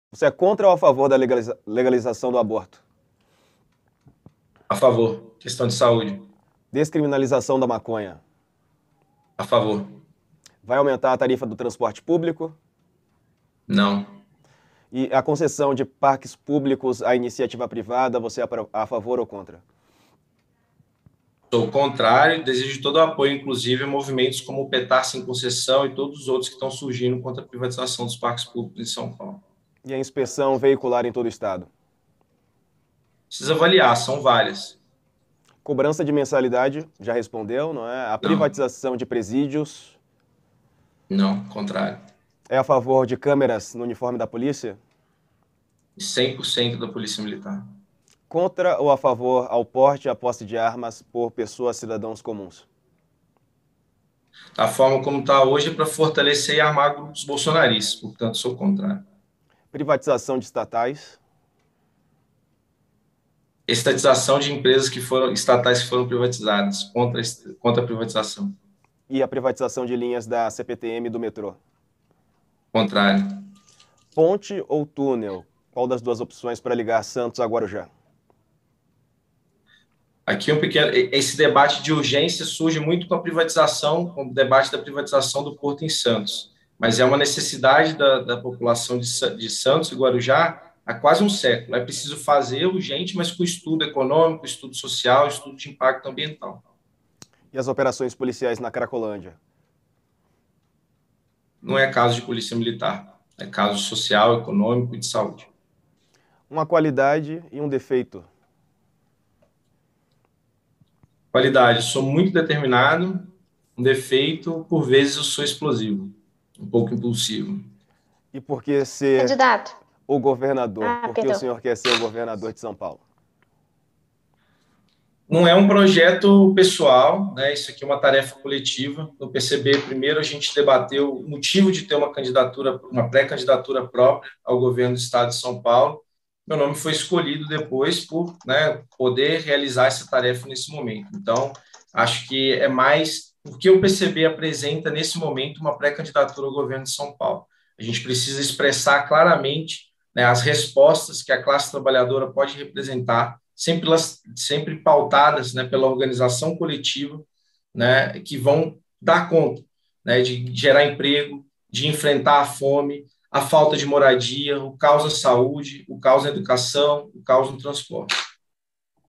Gabriel Colombo responde sobre aborto, maconha, cracolândia e mais
Pré-candidato ao governo de São Paulo pelo PCB, Gabriel Colombo é sabatinado nesta quinta (5).